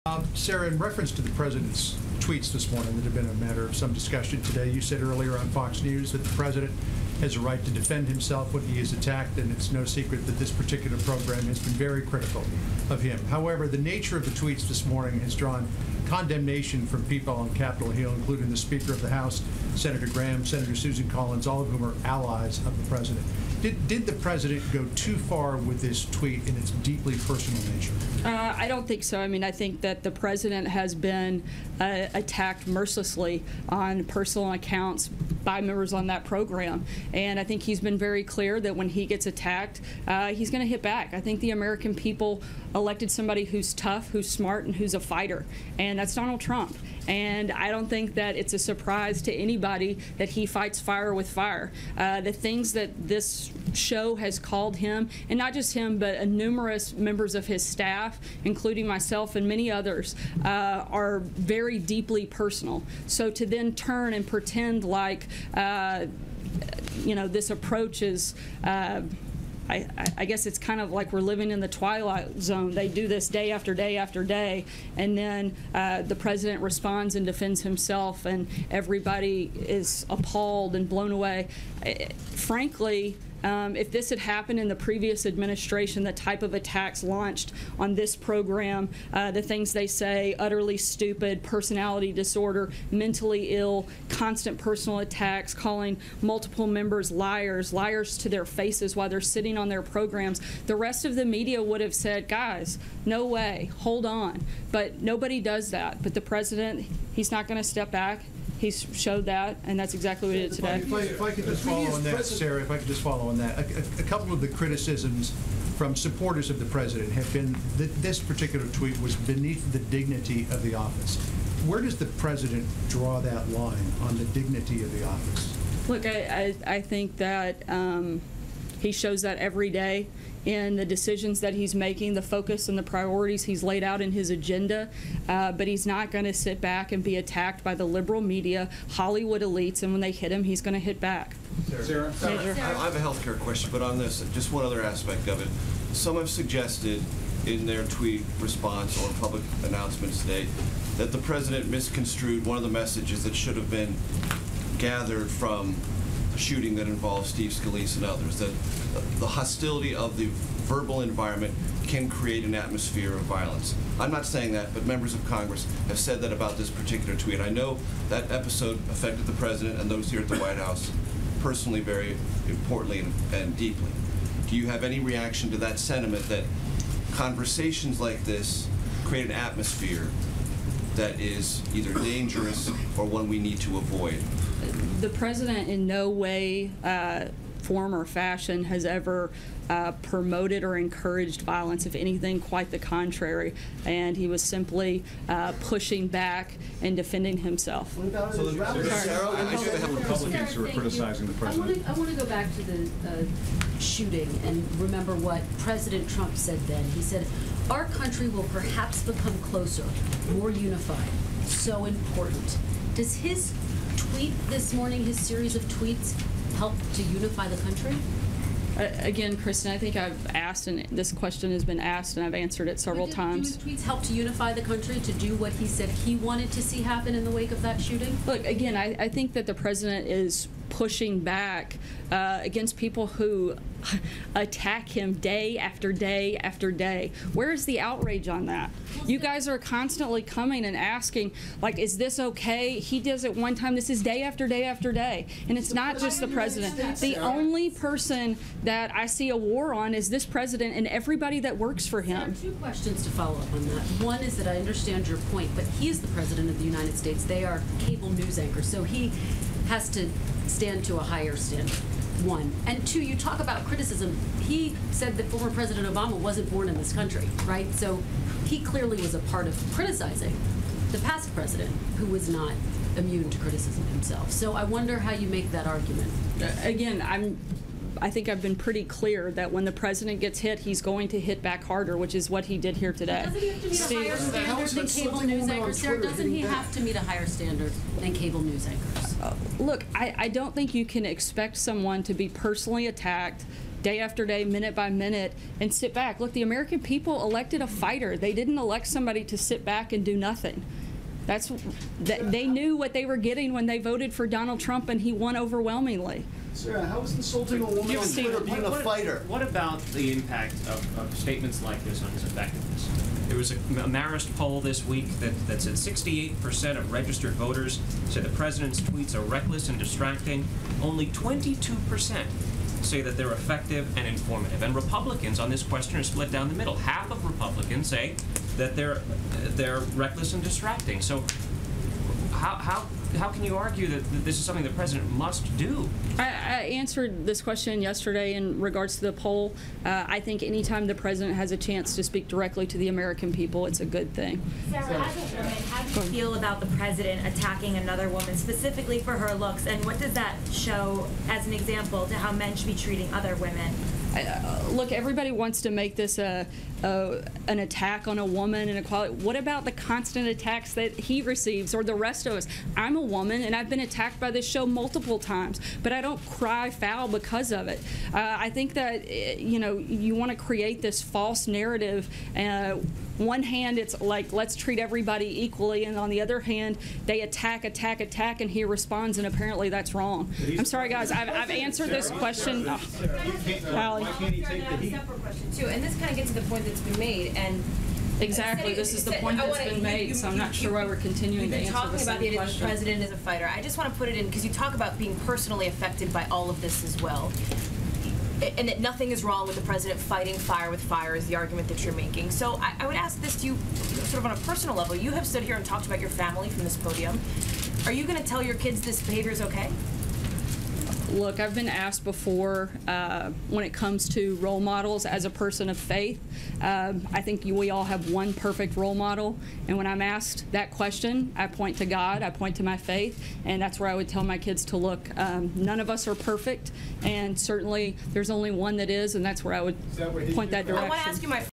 CLIP: Sarah Huckabee Sanders on President Trump's tweet about Mika Brzezinski​. (C-SPAN)
CLIPS: White House Principal Deputy Press Secretary Sarah Huckabee Sanders answers multiple question at the White House Press Briefing on President Trump's tweet about Mika Brzezinski.

Q: "Did the president go too far with this tweet in its deeply personal nature?
A: "I don't think so."